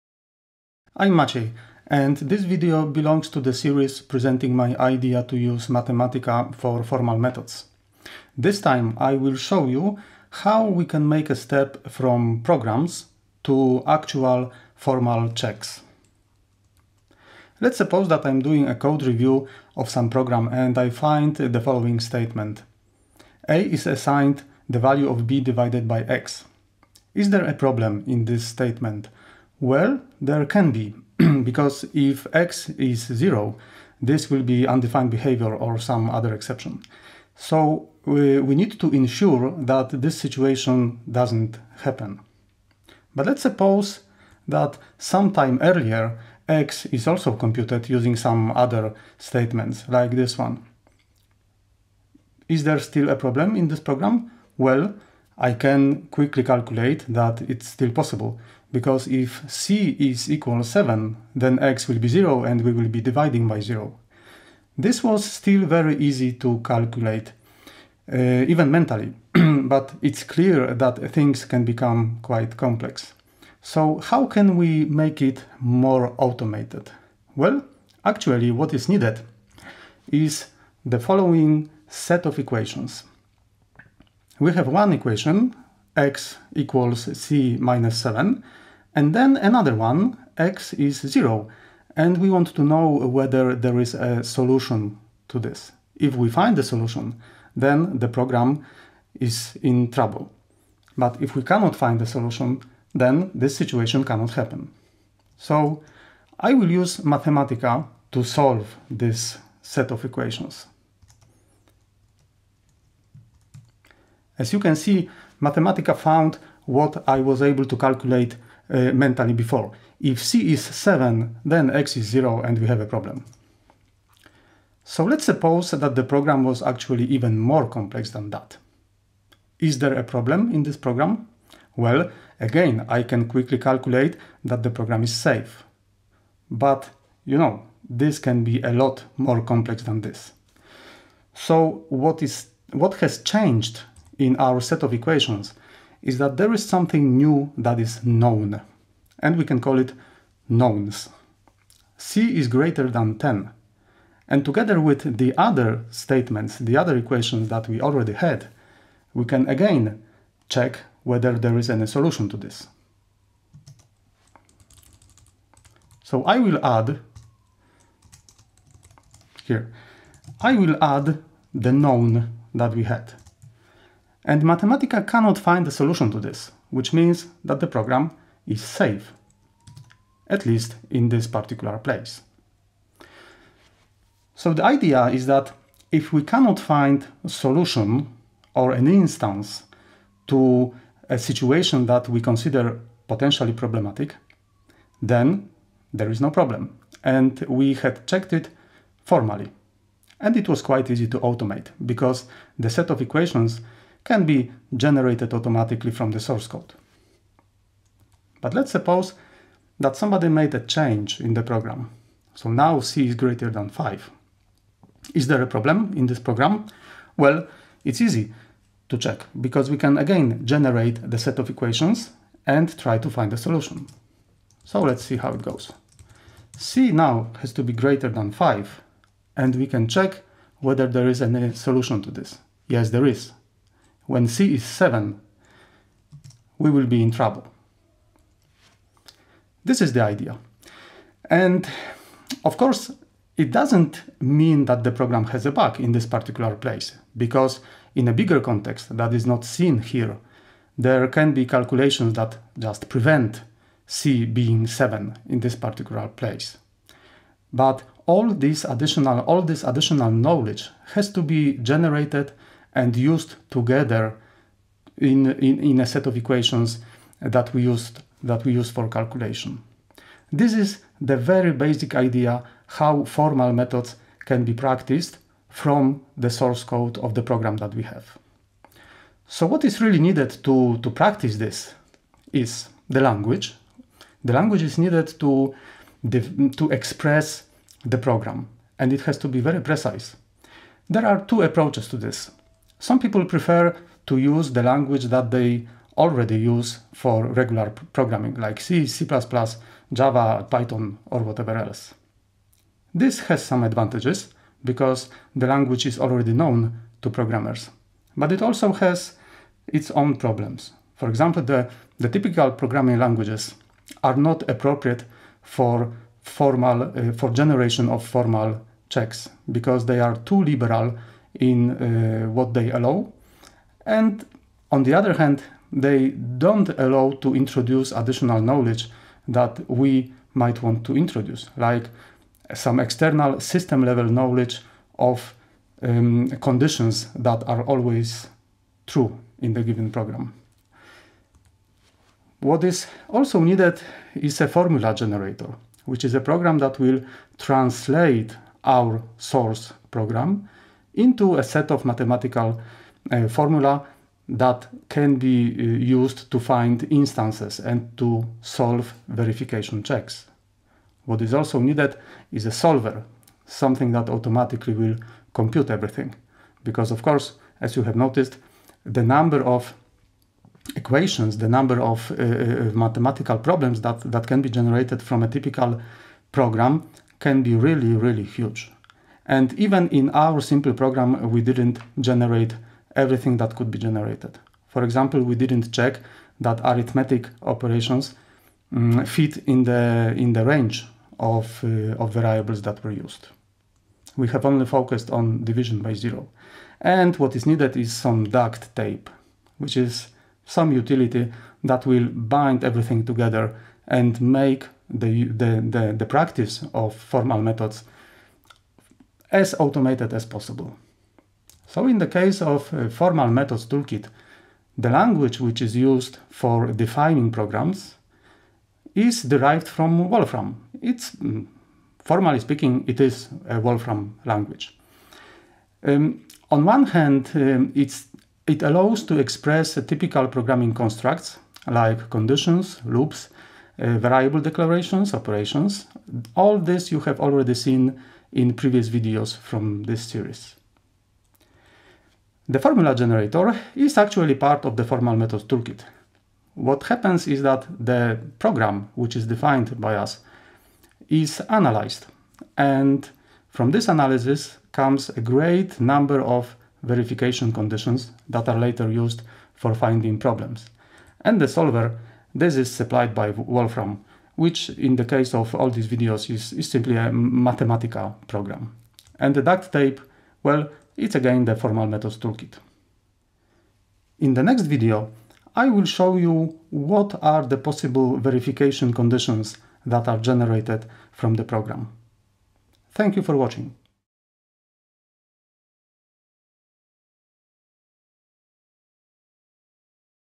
FMT 14 From Program To Formal Checks
The very basic idea of translating a program source code into a set of verification conditions - and an approach to automate this process.